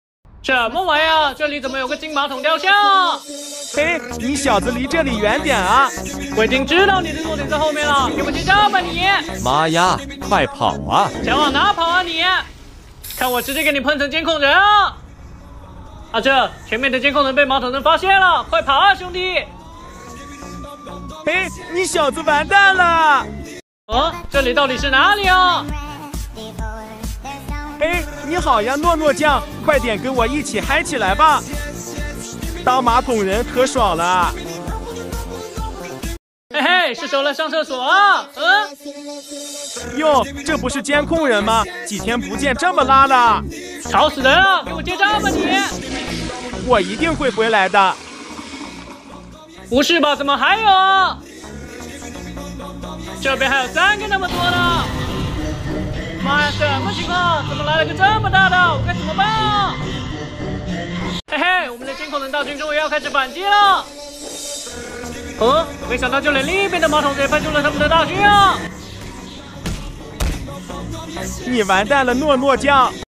Toilet Man VS Monitor: I buy it, I didn't expect the toilet man to become a DJ! ,Toilet Man vs. S
Toilet Man VS Monitor: I buy it, I didn't expect the toilet man to become a DJ!  ,Toilet Man vs. Surveillance Man ,I want to be a hit,original animation